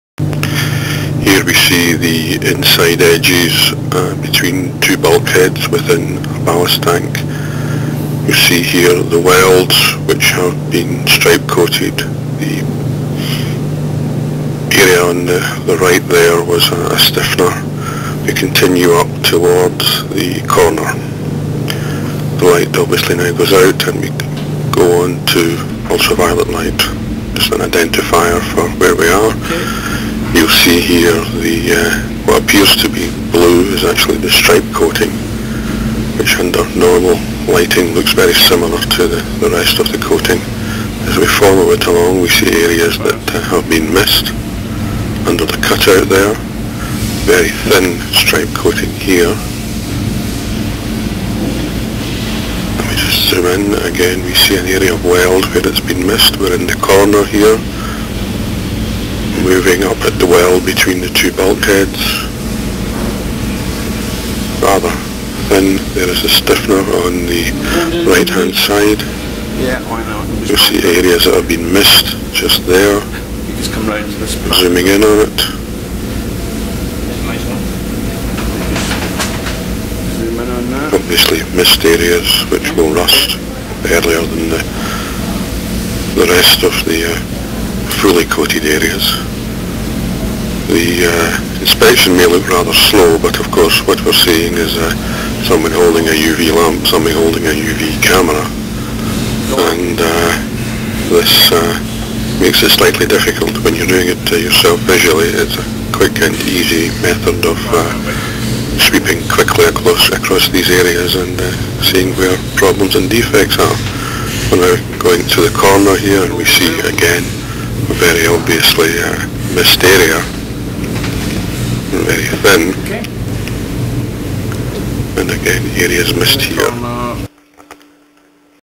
Short OAA Stripecoat Video with Audio
Video of real-time Optically Active Coating inspection in ballast tank.